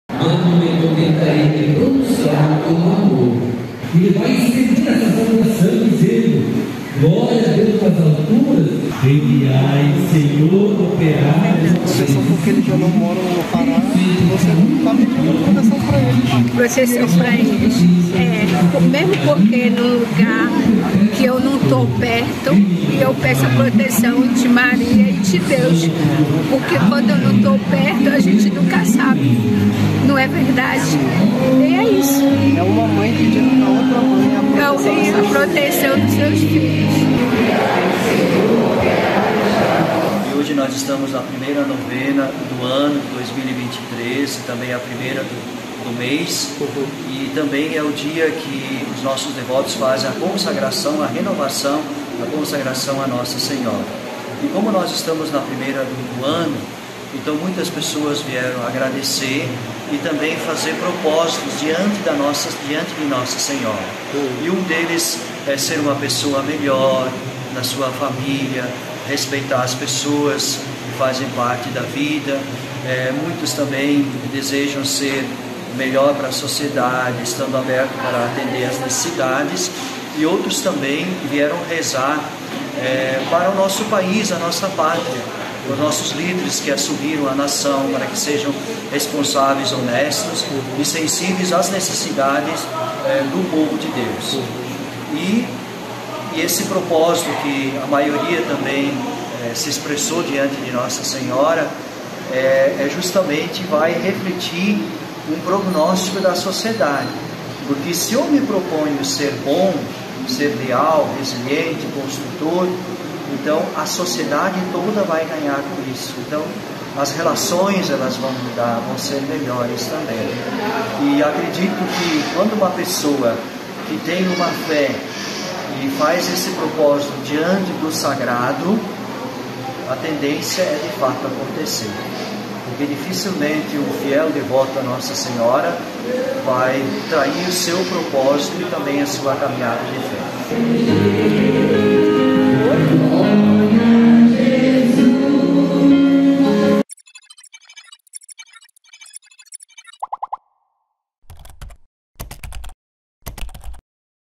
Primeira novena do ano de Nossa Senhora do Perpétuo Socorro movimenta devotos
A tradicional novena é realizada há mais de 70 anos, fundada pelos missionários redentoristas que administram o santuário, no bairro do Telégrafo. Fiéis e devotos de várias partes da Grande Belém apresentam diante da santa preces e pedidos para o início de um novo ano.